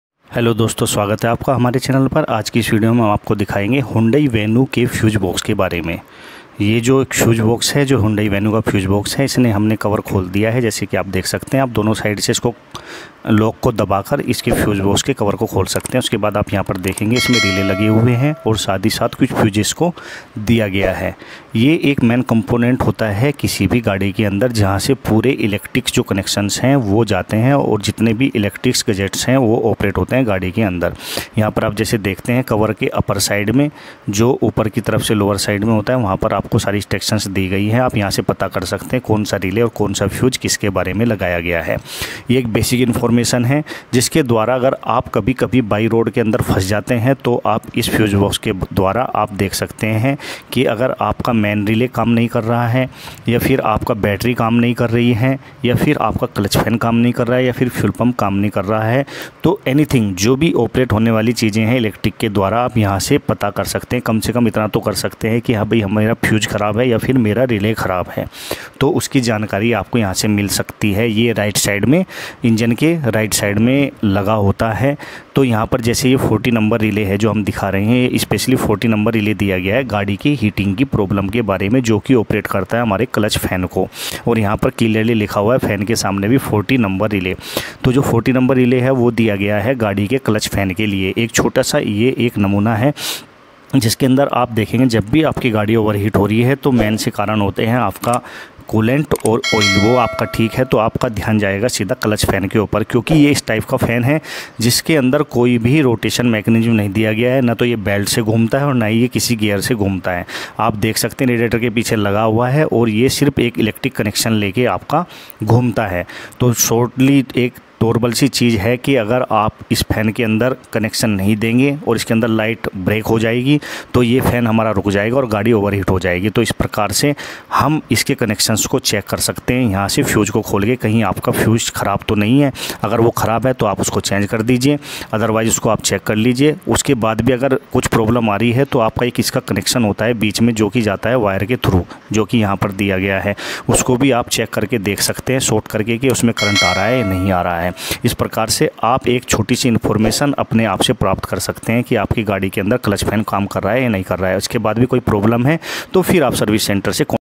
Hyundai venue Fuse Box गाड़ी की सभी समस्याएं यही, से शुरू यही पे खतम
This video is specially made for technical knowledge in which the fuse box used inside hyundai venu is explained in detail.What works in the fuse box used inside Hyundai and how to operate the connections, which fuse, which relay,  is made for which system, it is explained in this video as well as if the vehicle overheats.
Fuse box inside hyndai venue
Type of radiator fan used in venue
connections of fan
which relay and fuse faulty if fan not work
fault diagnosis